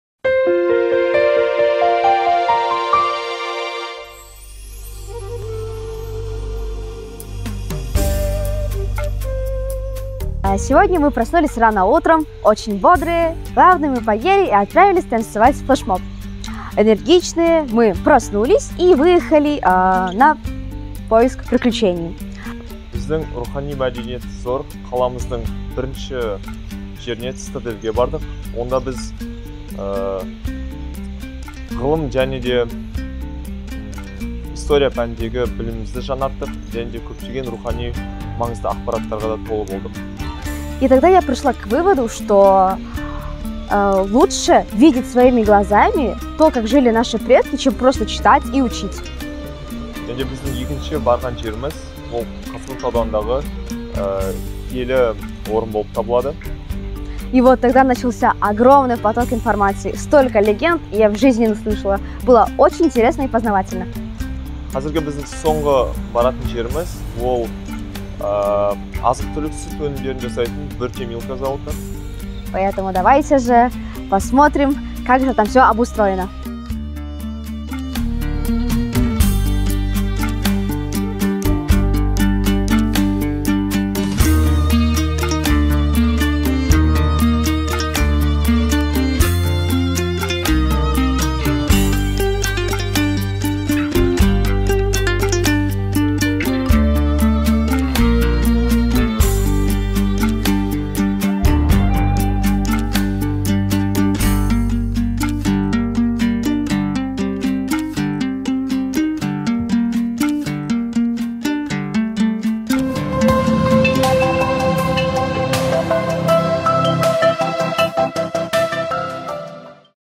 🔰ТУҒАН ЕЛГЕ ТАҒЗЫМ – 2021 2-КҮН: ЕСКІ ҚАЛАШЫҚҚА САЯХАТ
«Туған елге тағзым» өлкетану-зерттеу экспедициясының екінші күнінде Шымкенттік зияткерлер Шымкенттің жарқын туристік брендіне, қала тұрғындары мен қонақтары үшін айрықша орынға айналған ежелгі «Цитадель» қалашығы және «Қошқар ата» өзенінің жағалауына саяхаттап, «Ғайып ерен қырық шілтен» мәдени орнының тарихымен және  «Кемеқалған» монументіне Нұх пайғамбардың кемесі жайлы, Қазығұрт тауы жайлы аңыздар желісімен танысты.